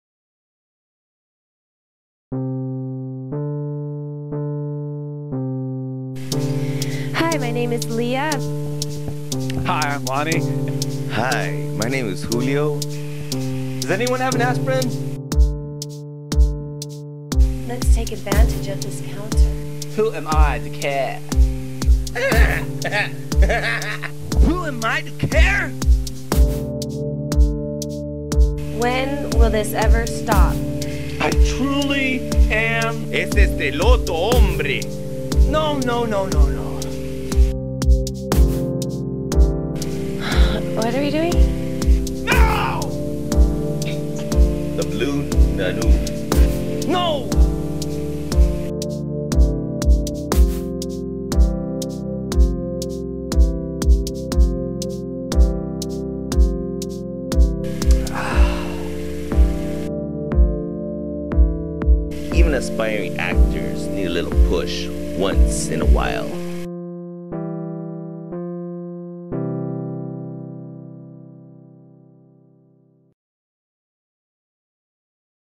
Something From Nothing
This is a short film about actors and the motivations they go through to make a commercial that is never made.  Staring Julio Alcala, Aurelio Ocampo Jr, Lea Baskas, and Julio Lonnie Lopez.  Shot on 8mm film.  Music by DJ RSLK.  Edited with Sony Vegas Video Pro.